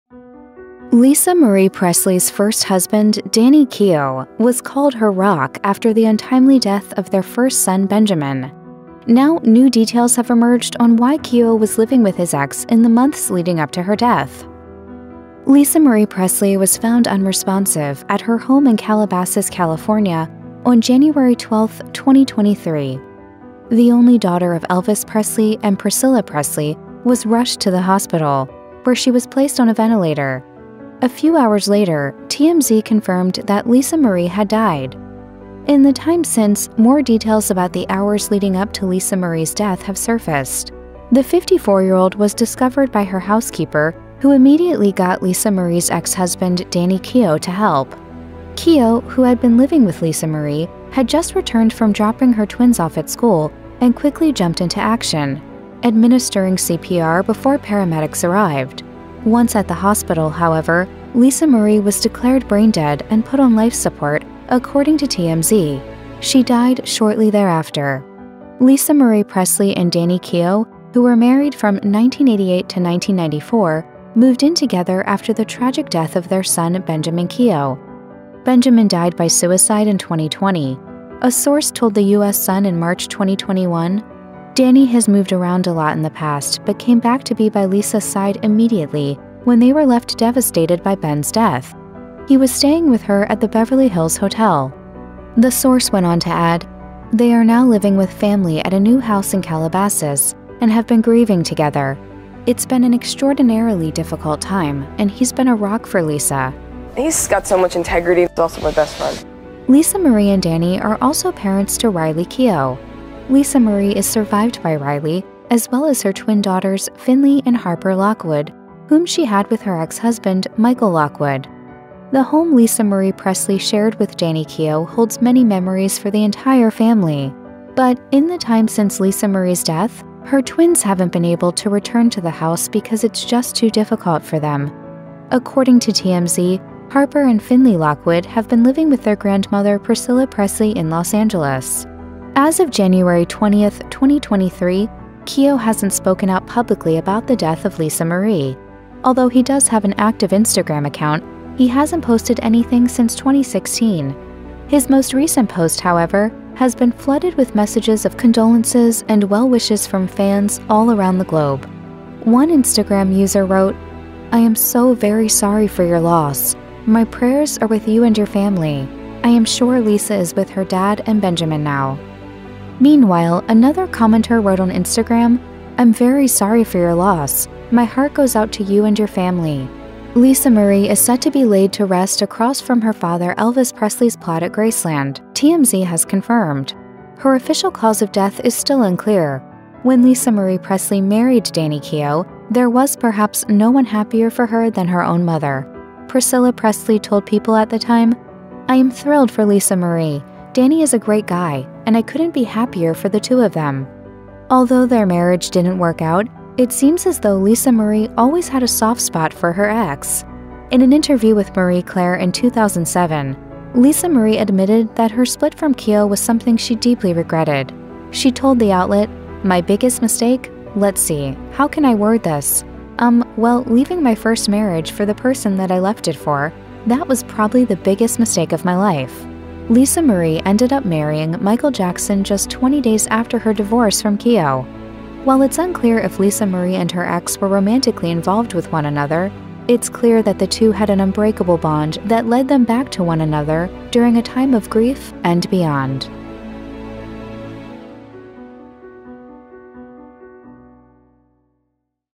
Why Lisa Marie Presley Lived With Her Ex Danny Keough After Their Split
Lisa Marie Presley's first husband, Danny Keough, was called her "rock" after the untimely death of their first son, Benjamin. Now, new details have emerged on why Keough was living with his ex in the months leading up to her death.